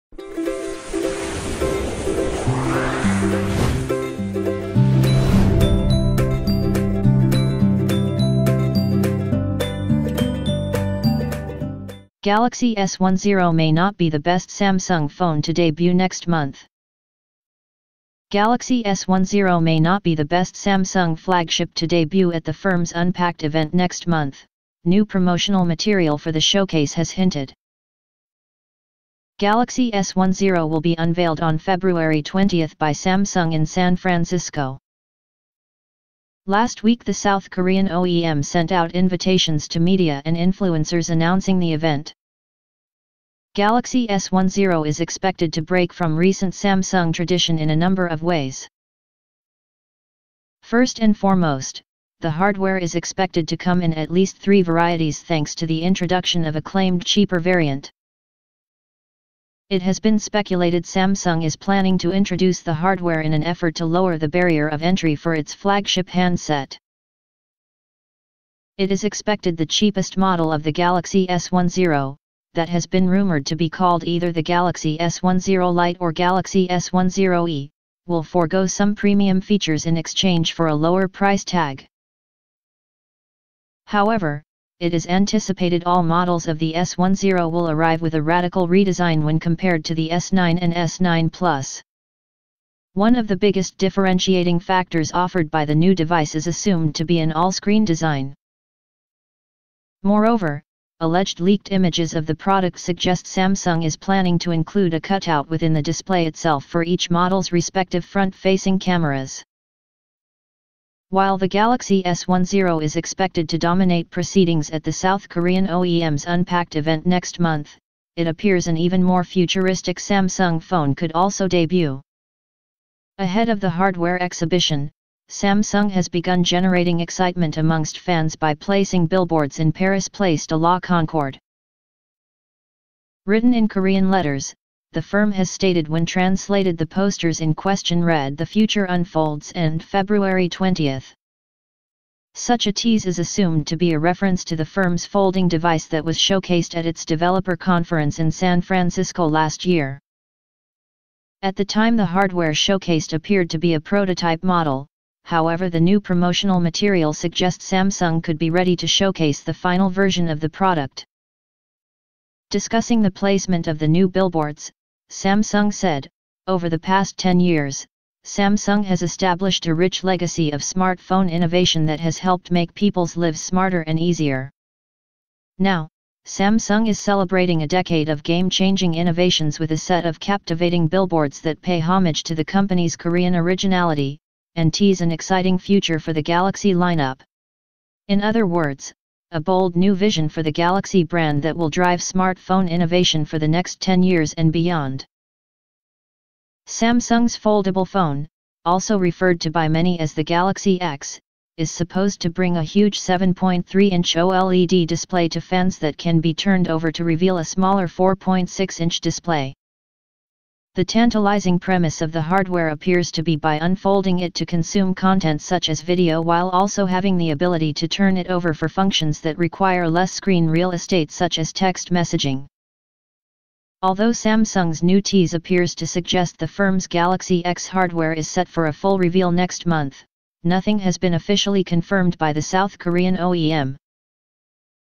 Galaxy S10 may not be the best Samsung phone to debut next month
Galaxy S10 may not be the best Samsung phone to debut next monthGALAXY S10 may not be the best Samsung flagship to debut at the firm's Unpacked event next month, new promotional material for the showcase has hinted.Galaxy S10 will be unveiled on February 20 by Samsung in San Francisco.Last week the South Korean OEM sent out invitations to media and influencers announcing the event.Galaxy S10 is expected to break from recent Samsung tradition in a number of ways.First and foremost, the hardware is expected to come in at least three varieties thanks to the introduction of a claimed cheaper variant.It has been speculated Samsung is planning to introduce the hardware in an effort to lower the barrier of entry for its flagship handset.It is expected the cheapest model of the Galaxy S10, that has been rumoured to be called either the Galaxy S10 Lite or Galaxy S10 E, will forgo some premium features in exchange for a lower price tag.However, it is anticipated all models of the S10 will arrive with a radical redesign when compared to the S9 and S9+.One of the biggest differentiating factors offered by the new device is assumed to be an all-screen design.Moreover, alleged leaked images of the product suggest Samsung is planning to include a cutout within the display itself for each model's respective front-facing cameras.While the Galaxy S10 is expected to dominate proceedings at the South Korean OEM's Unpacked event next month, it appears an even more futuristic Samsung phone could also debut.Ahead of the hardware exhibition, Samsung has begun generating excitement amongst fans by placing billboards in Paris' Place de la Concorde.Written in Korean letters, the firm has stated when translated the posters in question read “The future unfolds” and “February twentieth".Such a tease is assumed to be a reference to the firm's folding device that was showcased at its developer conference in San Francisco last year.At the time the hardware showcased appeared to be a prototype